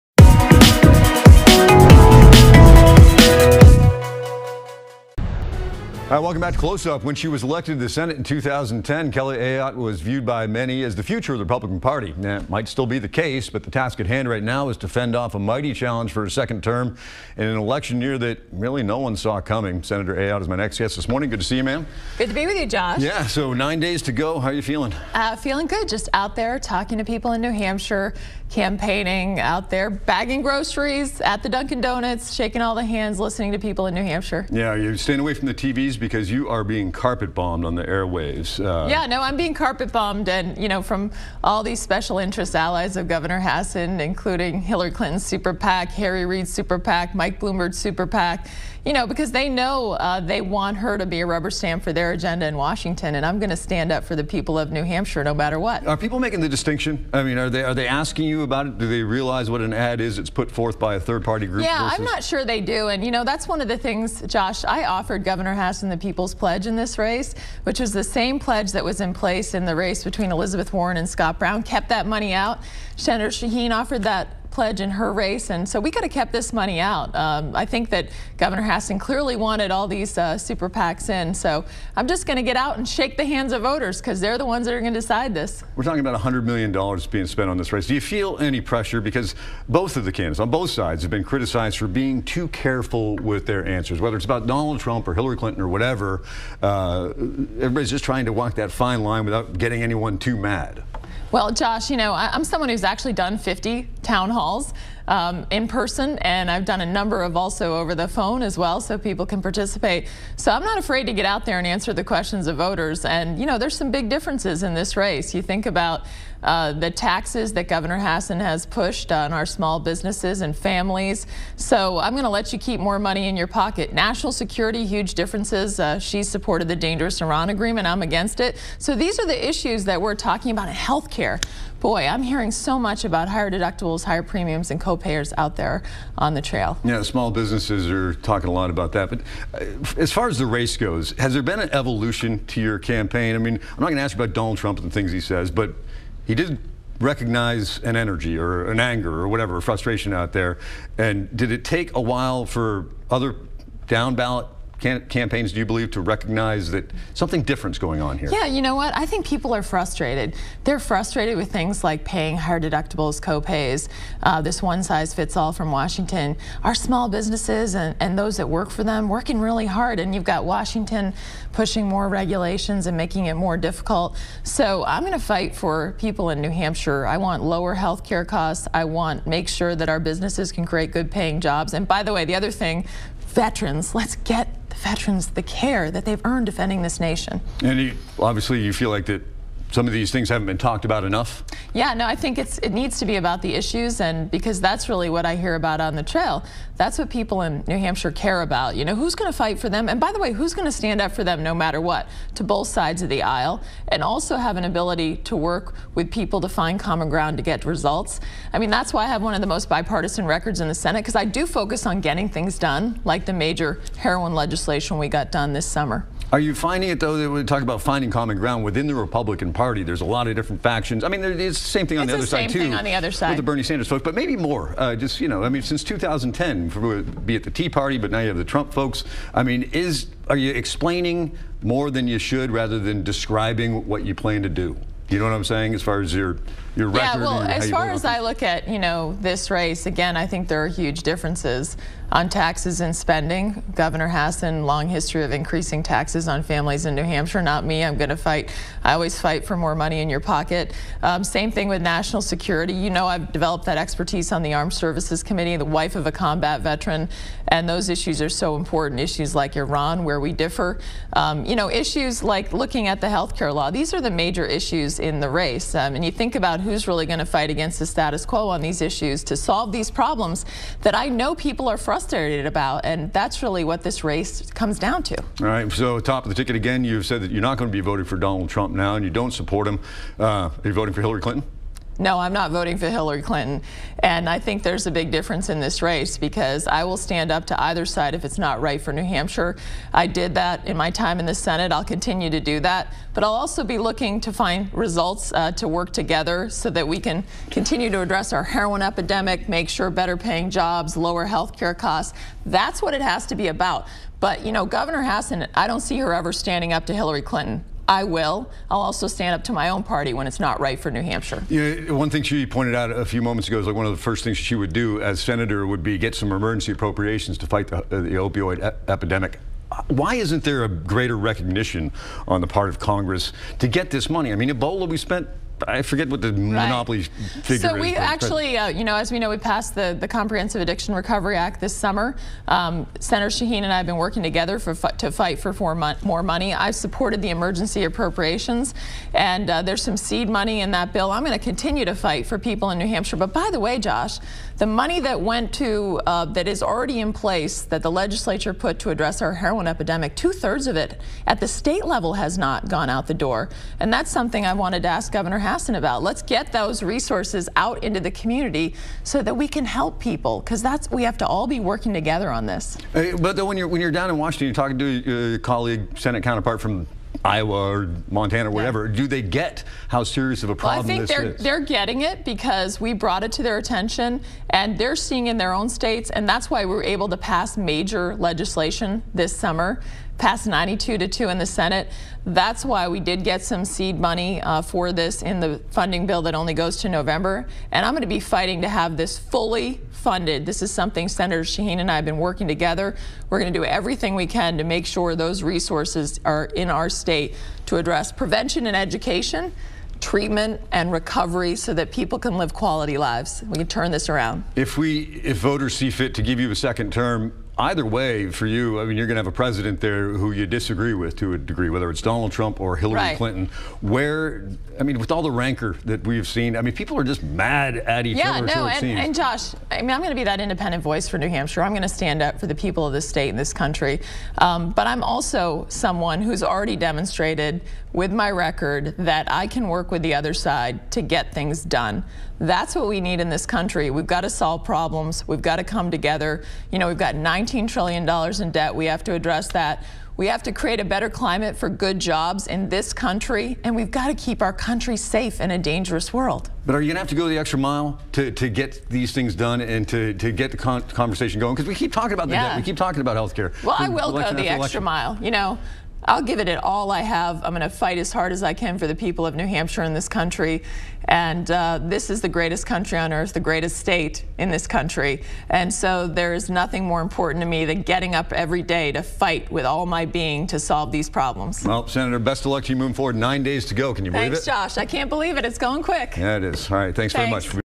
CloseUP: Kelly Ayotte on how she is connecting with voters
Kelly Ayotte talk attack ads, 3rd party money, and how she connects with voters in New Hampshire who are frustrated with Washington policies.